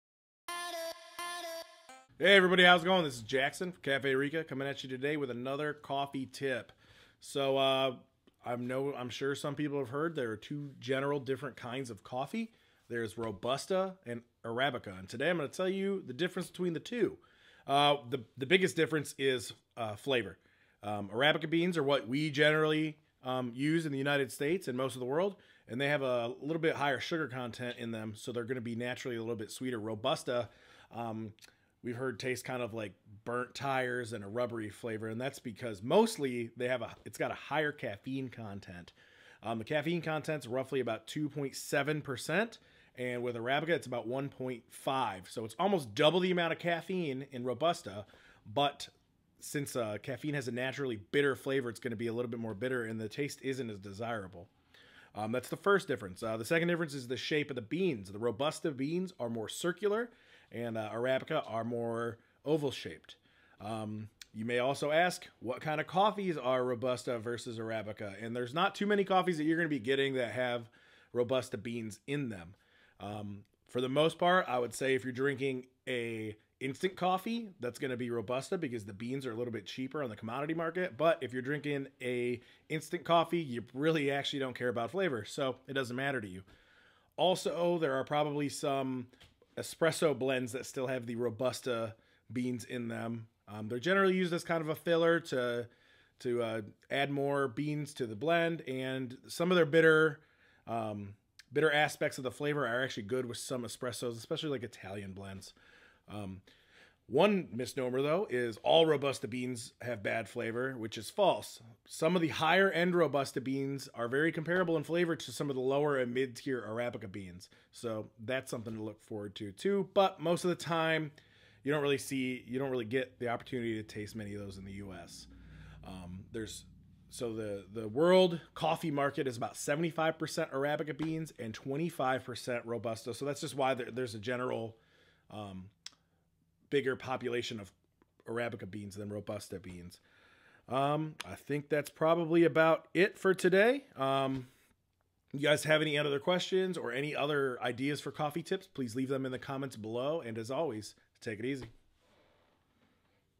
Arabica vs Robusta | Day 13 of 31 Days of Coffee
There are 2 types of coffee beans, Arabica and Robusta. They contain different amounts of caffeine and have different tastes. Jackson Bredehoft explains the difference between the two in today's daily tip

Day 13 of 31 Days of Coffee